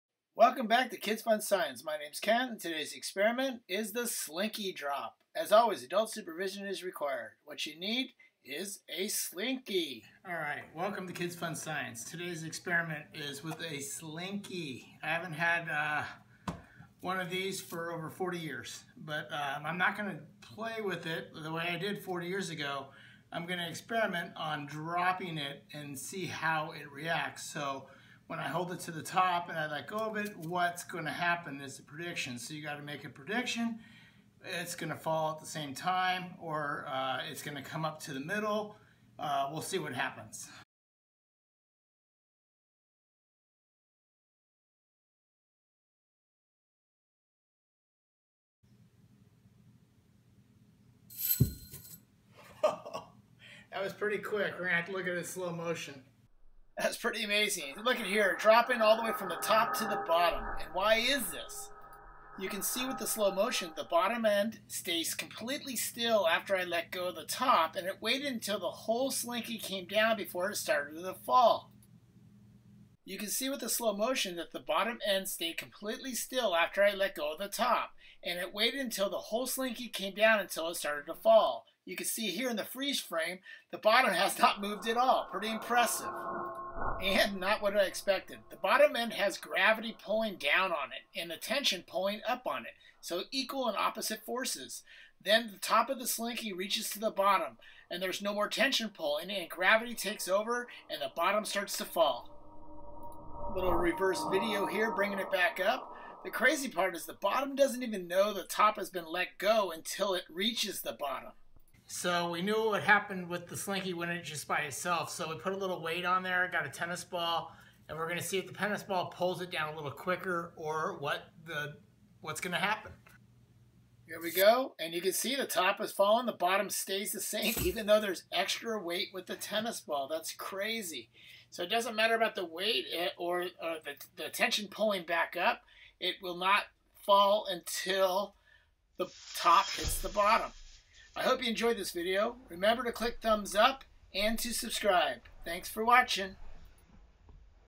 Slinky Drop Science Experiment
If i drop a Slinky you probably won’t be able to see it what happens, as it happens to fast. In Slow motion you can see the results, but maybe not understand what you saw.

You can see with the slow motion that the bottom end stayed completely still after I let go of the top and waited until the whole shiny came down until the it started to fall.

The bottom end has gravity pulling it down and tension pulling it up. Equal and opposite forces, then when the top of the slimy reaches the bottom  there is no more tension pulling and gravity takes over and the bottom starts to fall.

The bottom does not even know that the top has been let go until it reaches the bottom.

This is not what I expected for the results. Very fun experiment. Let me know you results in the comments below?

HELP! ME TRANSLATE my Videos!! aTdHvAaNnKcSe